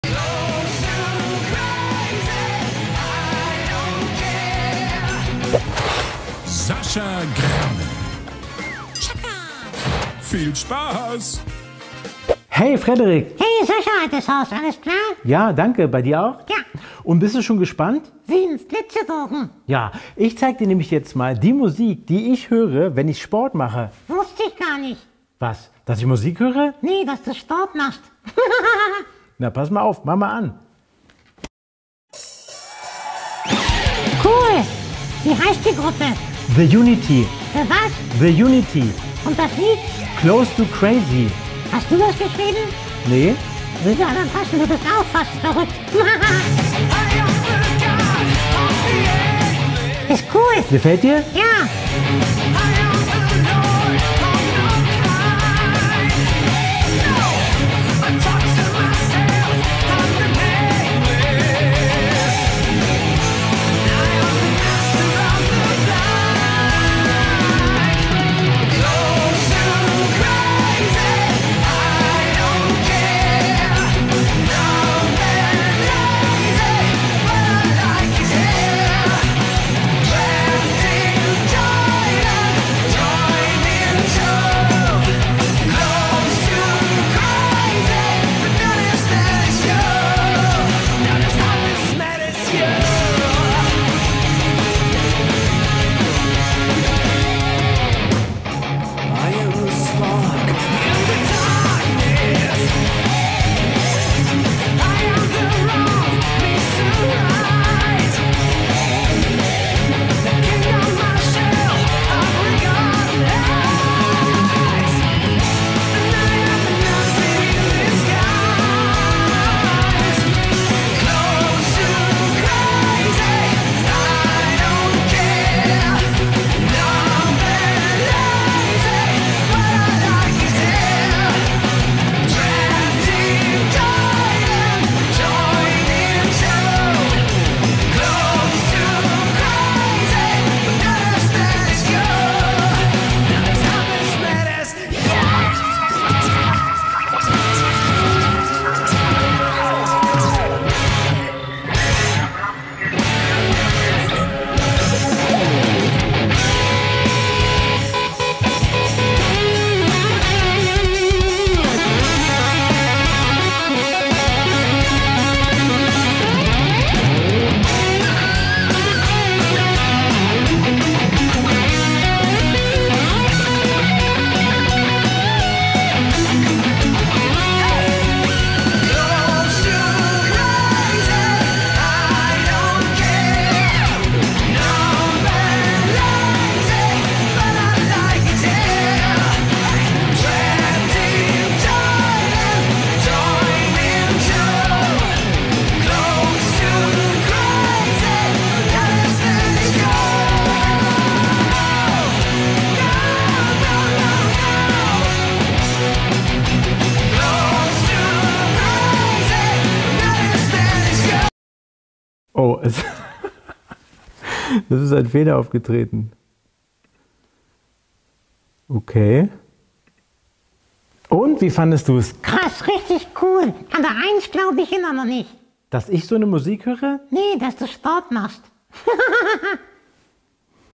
Sascha Grammel feat. The Unity!
Frederic rockt richtig ab und kann es nicht glauben!

Sascha Grammel ist ein deutscher Comedian, Puppenspieler und Bauchredner.